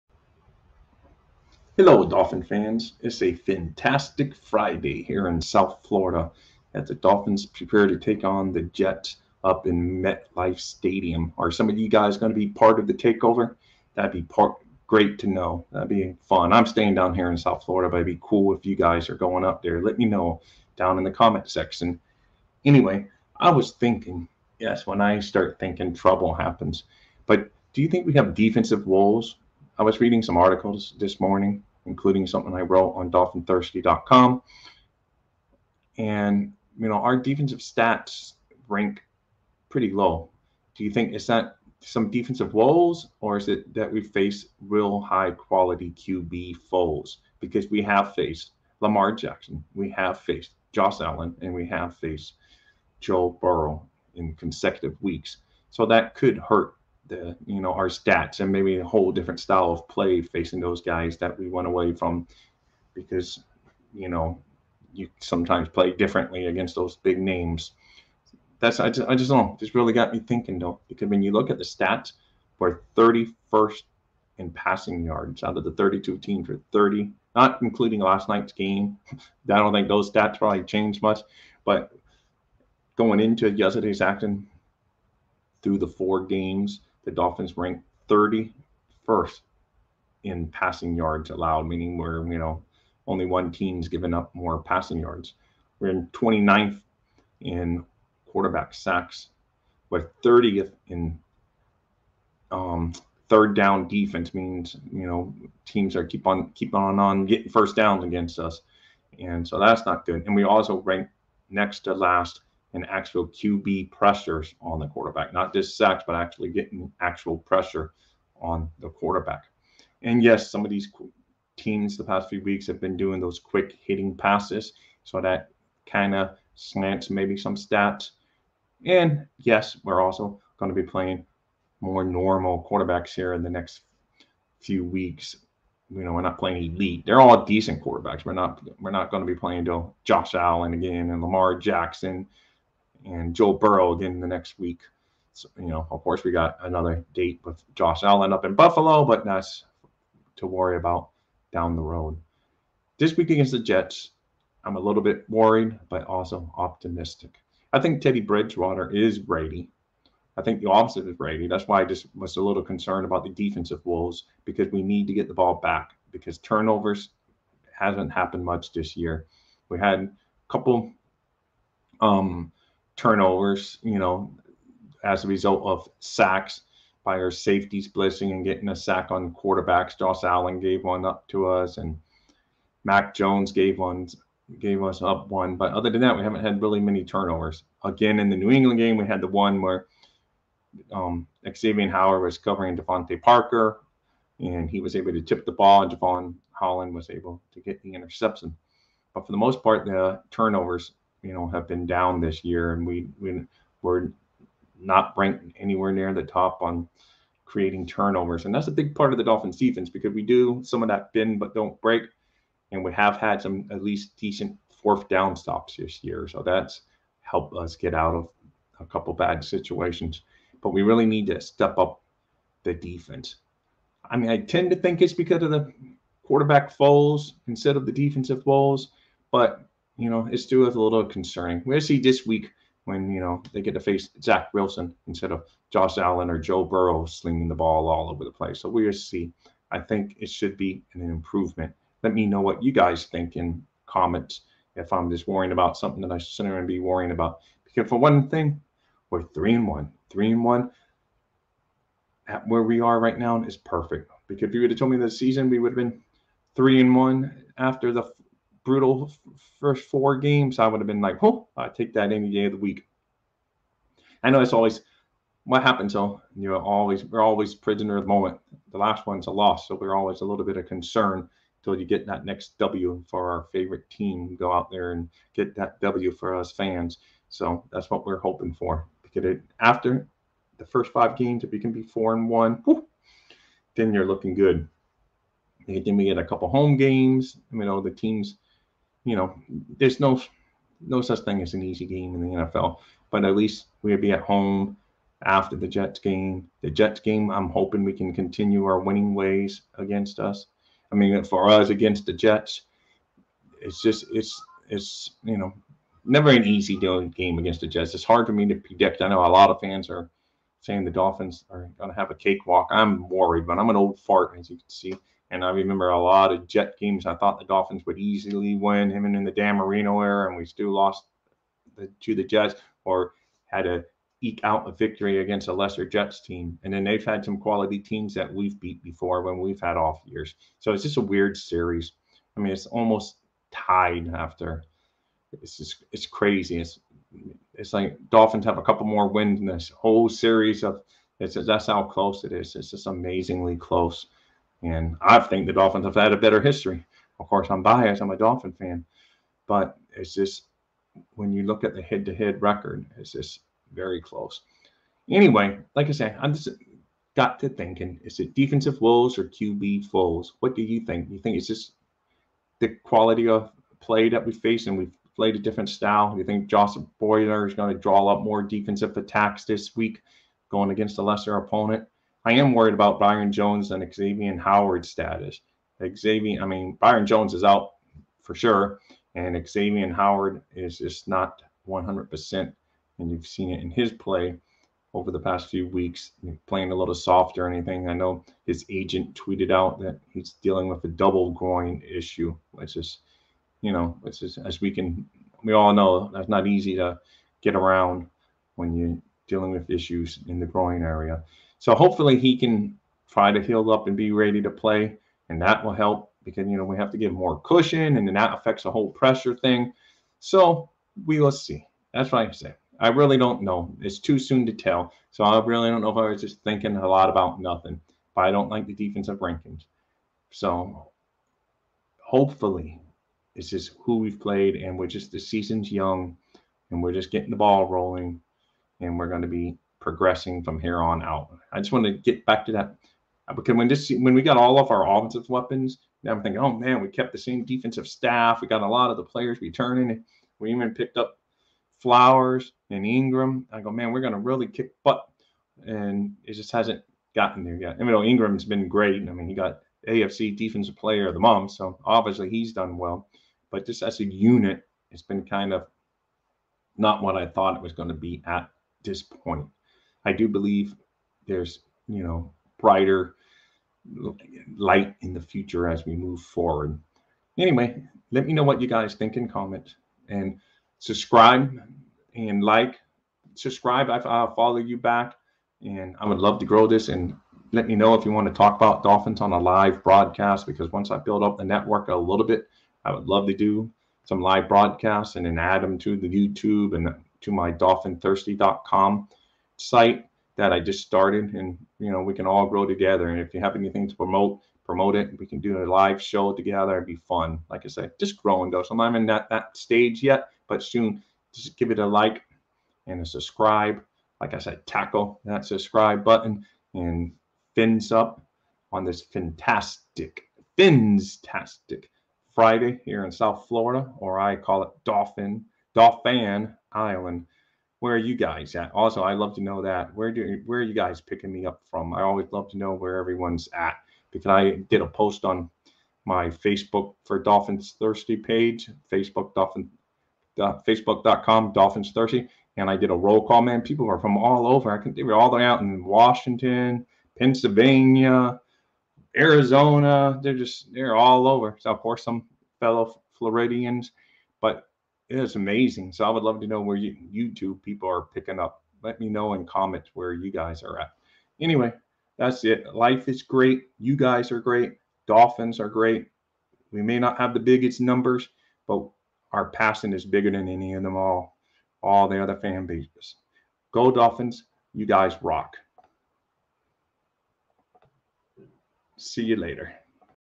Is it Defensive Woes or QB Foes? Miami Dolphins VLOG
Do the Miami Dolphins have defensive woes or has been the QB foes? So do you think the woes will lessen when the quality of QB foes decreases?
Will the defensive game plan change? A Miami Dolphins VLOG!